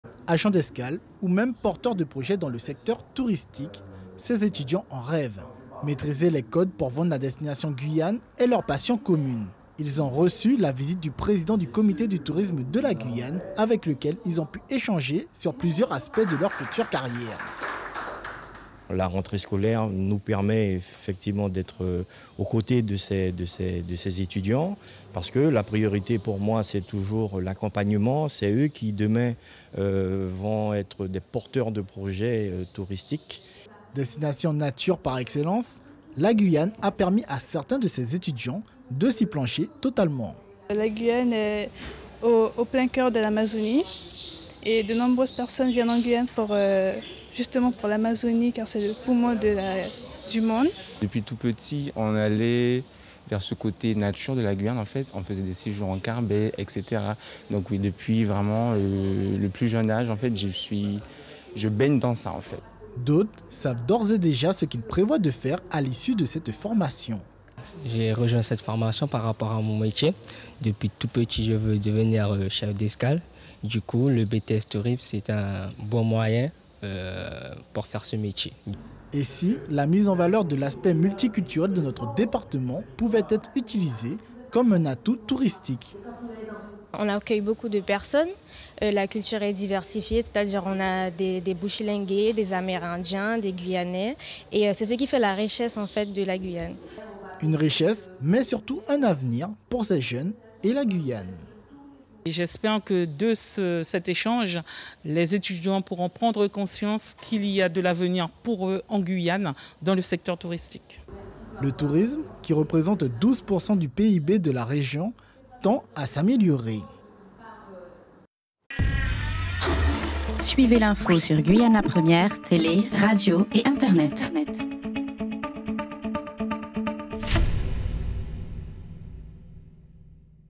Bts tourisme : les clés pour promouvoir le territoire
Ils auront la tache de valoriser la destination Guyane. Les étudiants en Bts tourisme du lycée Gaston Monnerville de Kourou ont fait leur rentrée la semaine dernière. Durant 2 ans ils vont appendre les clés pour vendre notre territoire aux résidents et au touristes.